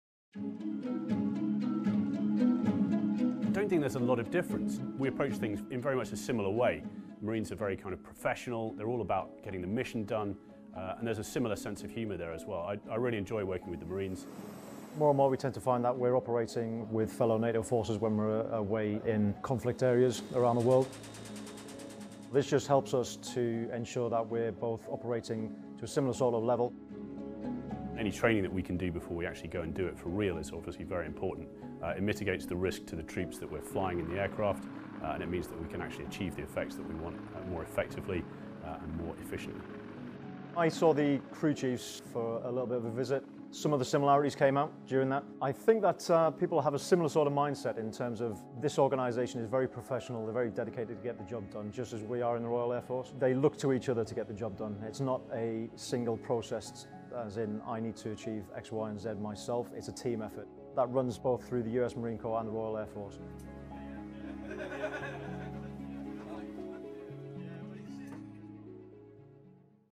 Sky's the Limit | Marines and Royal Air Force: Training Partners
When Marines come together with the Royal Air Force the the sky’s the limit.

(U.S. Marine Corps video by Sgt. Takoune H. Norasingh)